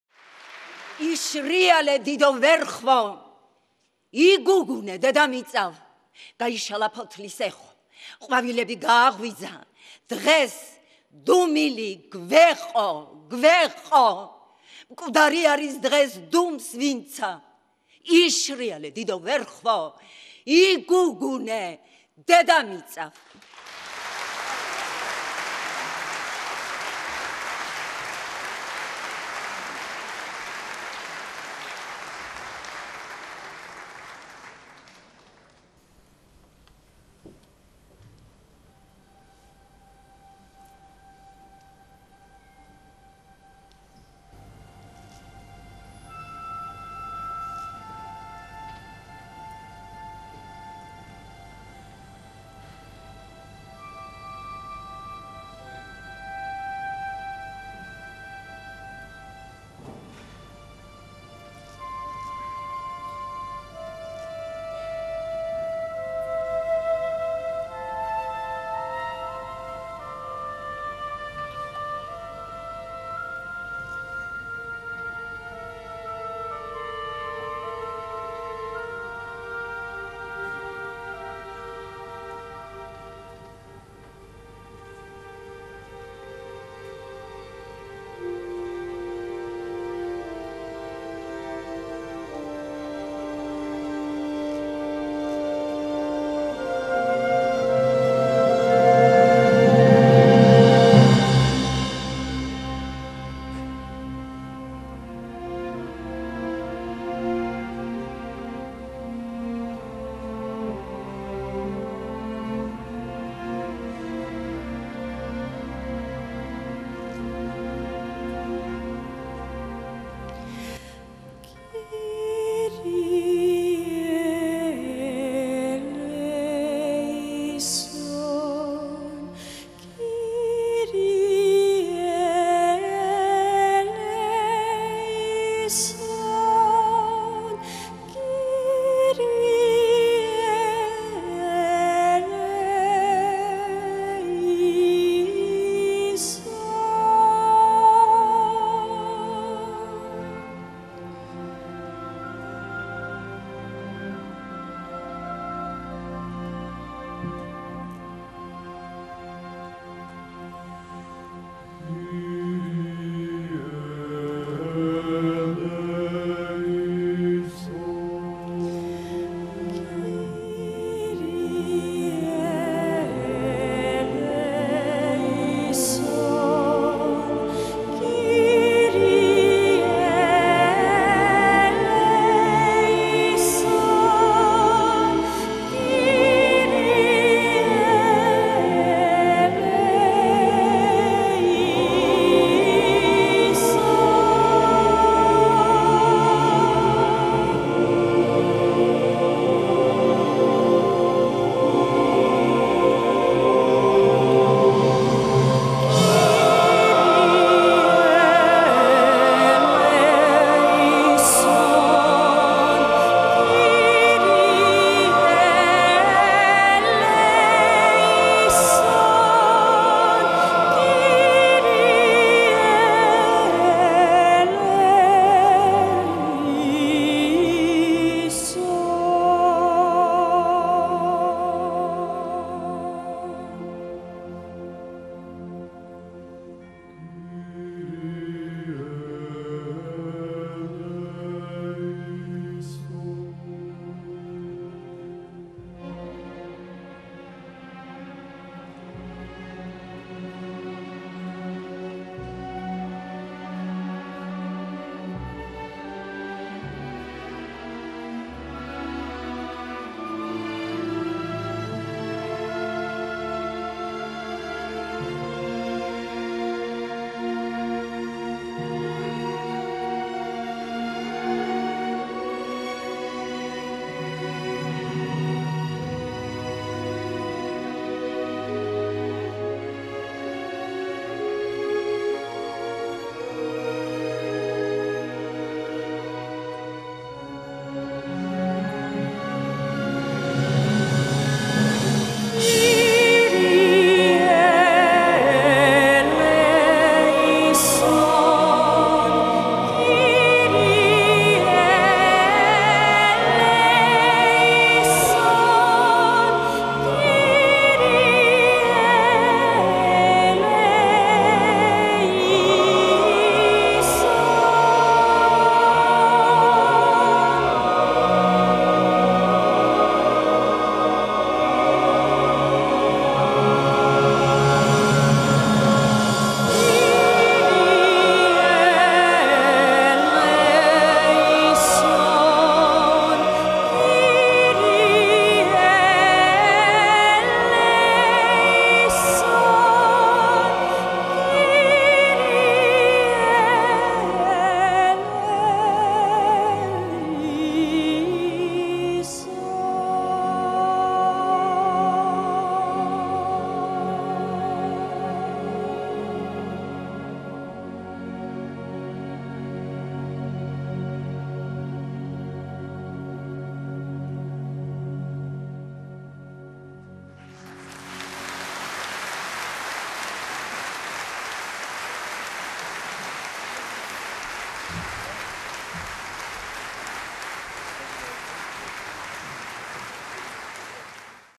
Zinaida Kverenchkhiladze / ზინაიდა კვერენჩხილაძე
Charity Foundation Iavnana's Gala Concert at Tbilisi Opera and Ballet Theater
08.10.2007

Trinity Cathedral Choir - Art Director: Svimon Jangulashvili & Giorgi Donadze

Screenwriter & Producer -  Teona Jorbenadze

ფონდ "იანანას" საქველმოქმედო გალა- კონცერტი თბილისის ოპერისა და ბალეტის თეატრში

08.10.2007

სრულიად საქართველოს კათოლიკოს პატრიარქის უწმინდესისა და უნეტარესის ილია II აღსაყდრების 30 წლისთავისადმი მიძღვნილი საქველმოქმედო საღამო.

"ის რაც ვიწამე"

სამების საკათედრო ტაძრის გუნდი - ხელმძღვანელი: სვიმონ ჯანგულაშვილი & გიორგი დონაძე

სცენარის ავტორი და სამხატვრო ხელმძღვანელი - თეონა ჯორბენაძე